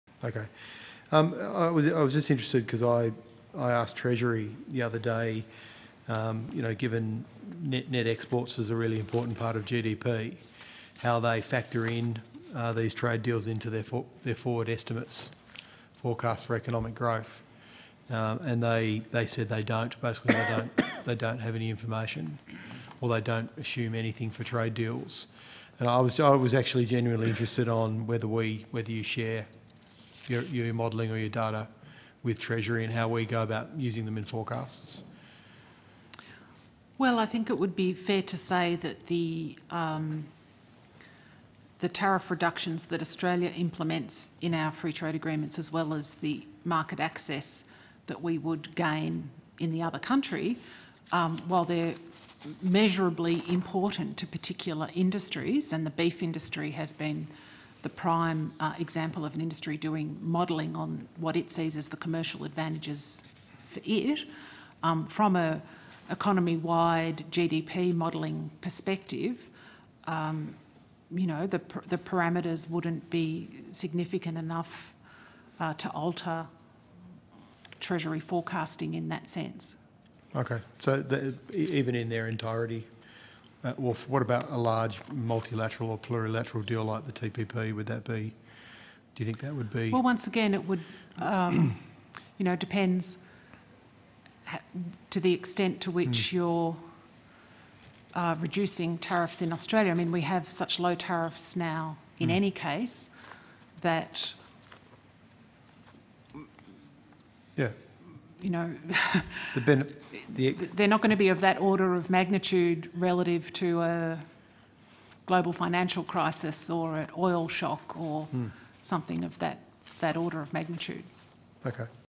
Will Abbott's trade deals like the TPP grow the economy? Not really.
Senate Estimates. Foreign Affairs, Defence and Trade Committee. Greens spokesperson for Trade asks DFAT if they work with Treasury to model the impact on the economy of their trade deals.